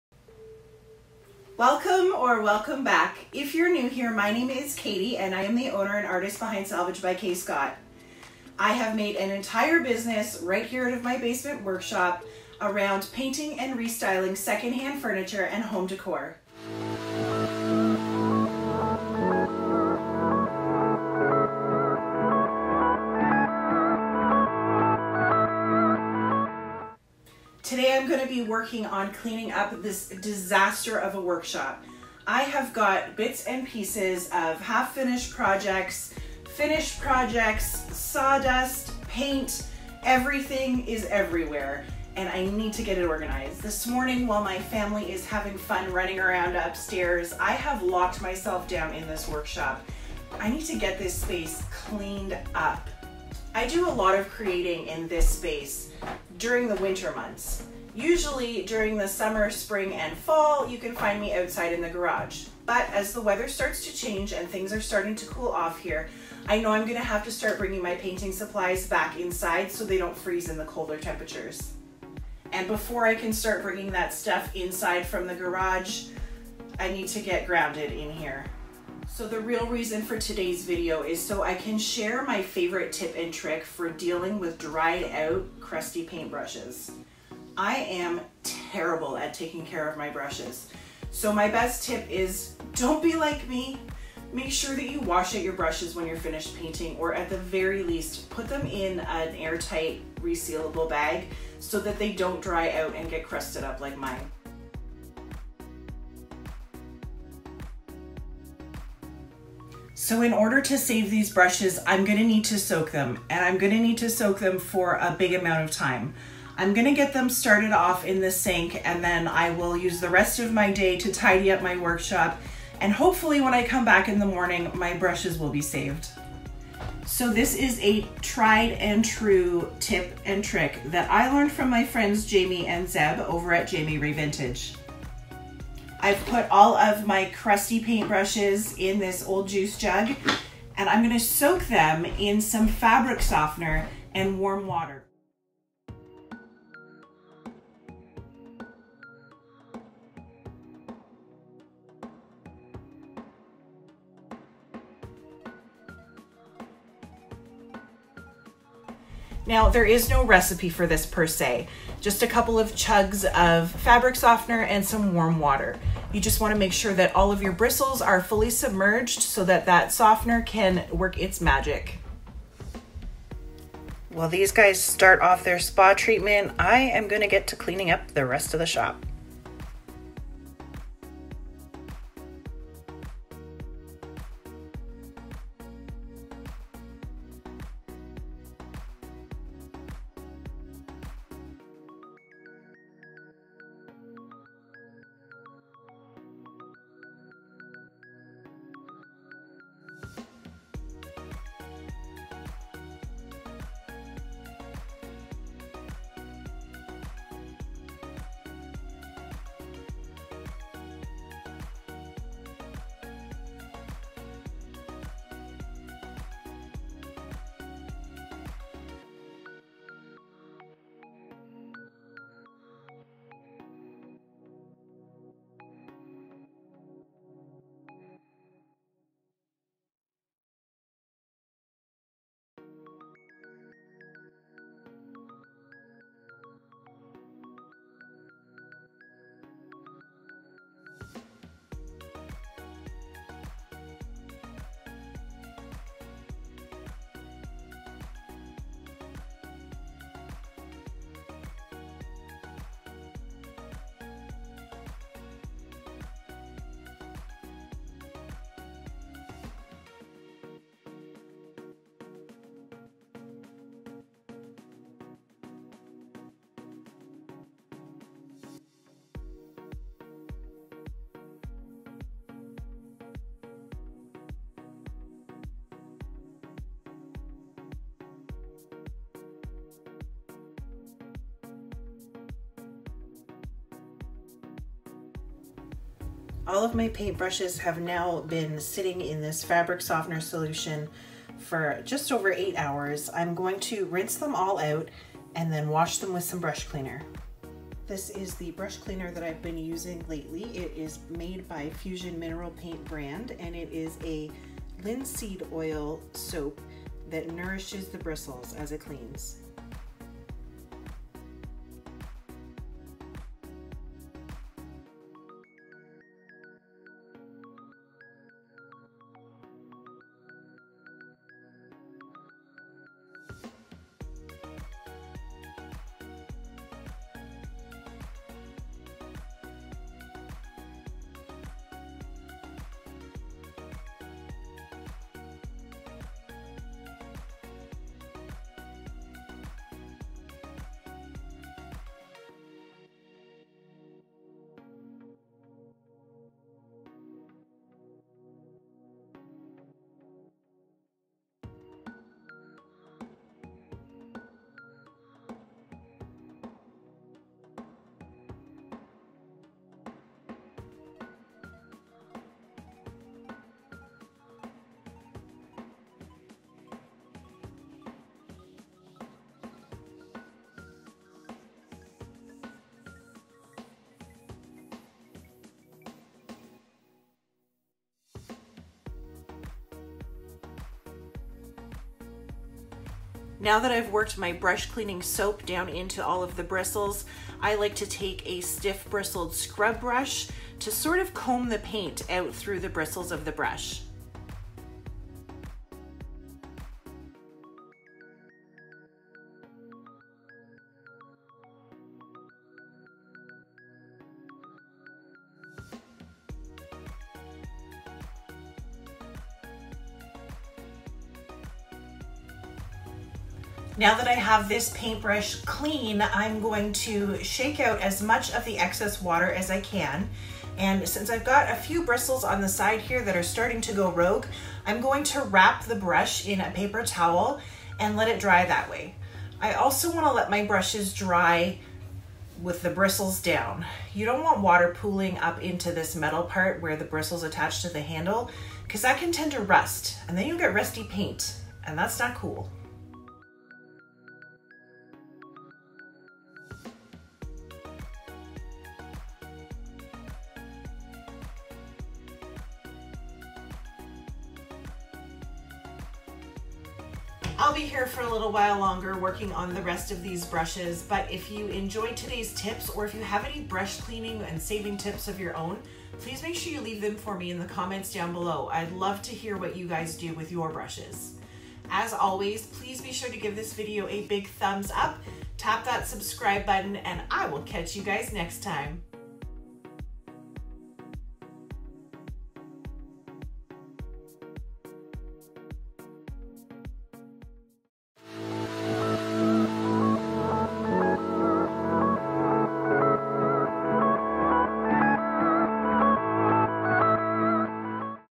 Brush Care | Workshop CLEAN WITH ME
I am TERRIBLE at taking care of my paint brushes! Brushes should always be washed out when you’re finished painting but I have a very bad habit of tossing in the sink and forgetting about them for days (or weeks).

In this video I’m sharing my favourite tip for softening up crusty bristles and getting my brushes ready to paint again. I learned this brush hack from my friends Jami & Zeb at Jami Ray Vintage.

Products Used Today:
Downy Fabric Softener
Fusion Brush Cleaner